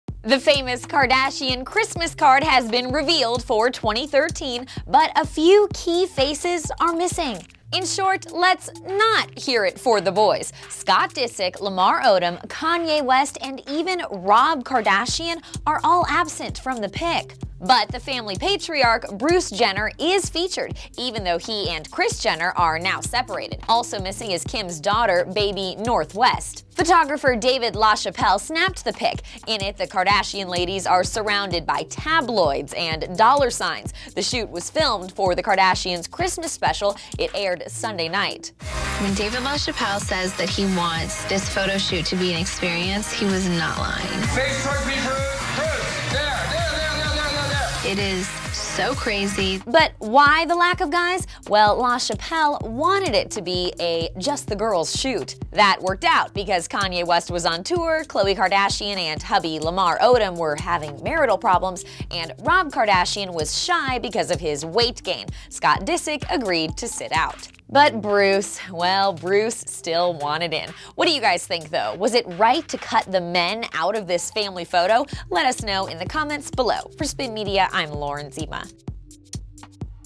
The Kardashian Christmas Card Is Revealed
The Kardashians revealed their annual holiday card on last night's Keeping Up With the Kardashians: a Very Merry Christmas, and this year it was taken by surrealist photographer David LaChapelle.

The men of the Kardashian family were represented only by Bruce Jenner however, as Rob Kardashian, Scott Disick, Kanye West and Lamar Odom were all absent from the photo. Also missing is Kim and Kanye's daughter North West.